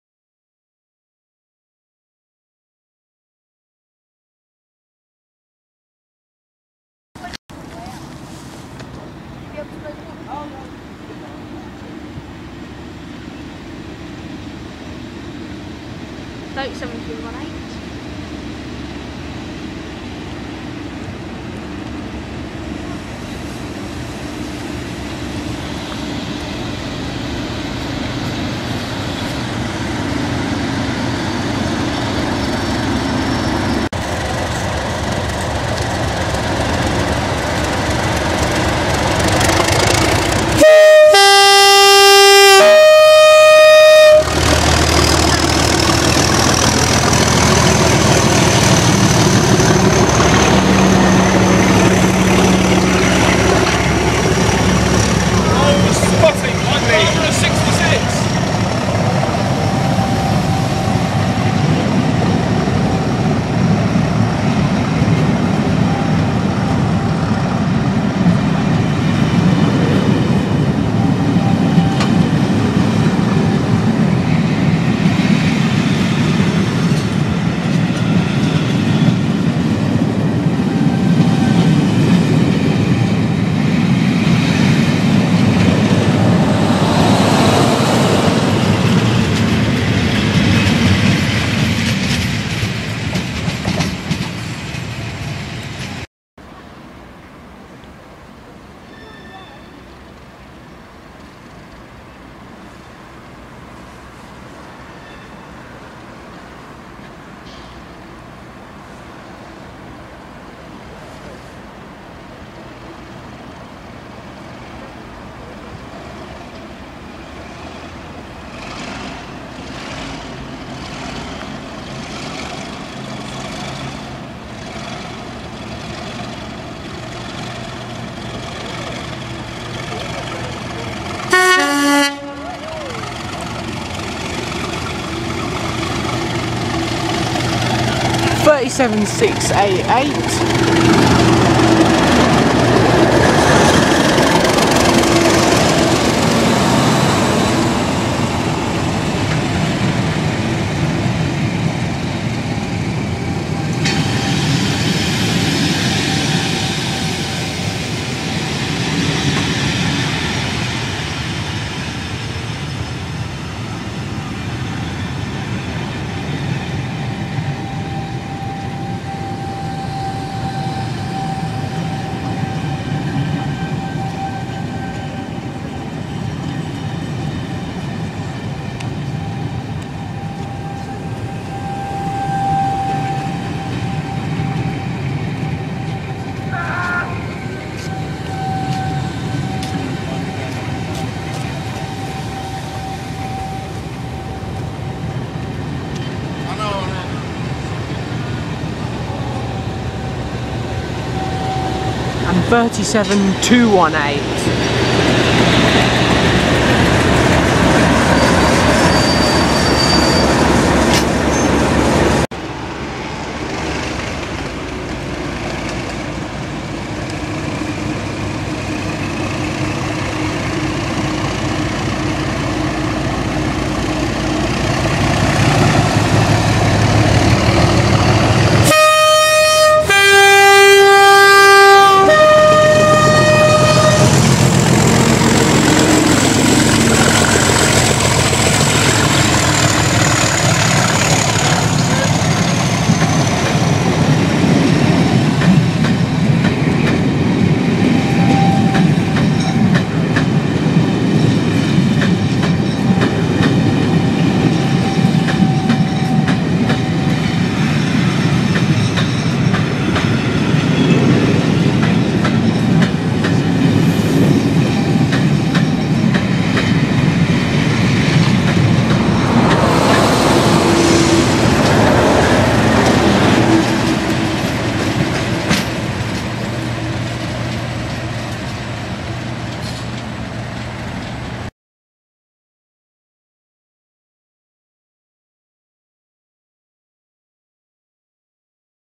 (HD) 37218+37688 Stafford to Portsmouth Harbour @ Eastleigh on 29/11/2014 with lots of thrash!
Awesome! This is the first DRS tour I have seen. This is the first time I have seen either of the locomotives and it is the first time I have seen the new DRS livery! :).